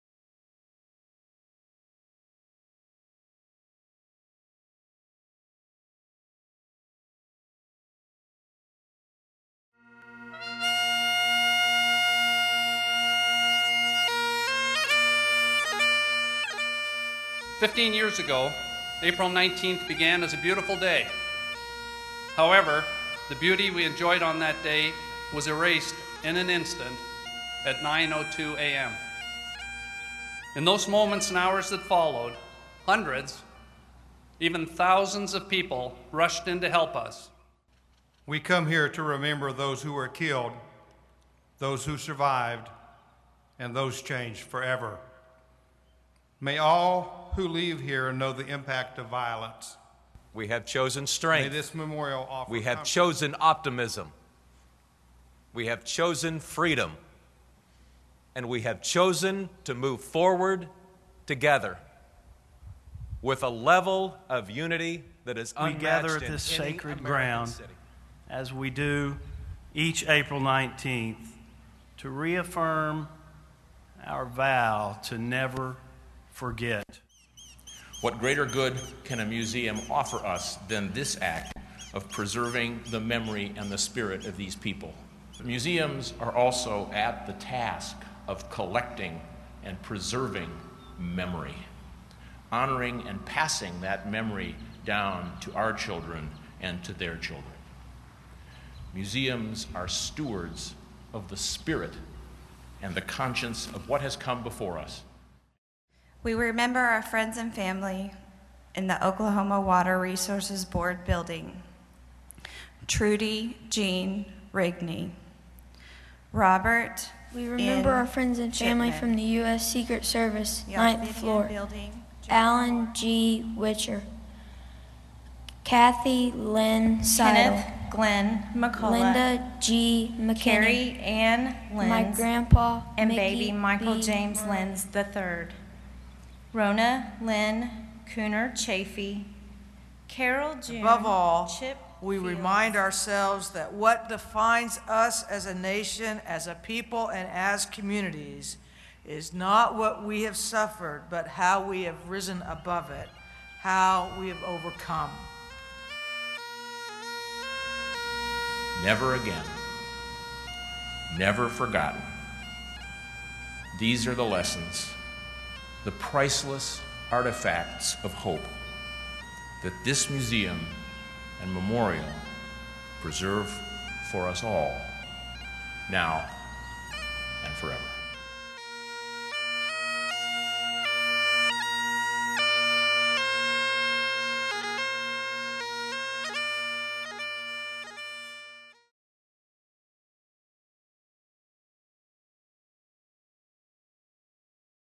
15th Anniversary Remembrance Ceremony
This year marks the 15th Anniversary of the day the world began to look at terrorism differently. On this important day, Monday, April 19th, the Remembrance Ceremony began at 9:00 a.m. on the grounds of the Memorial.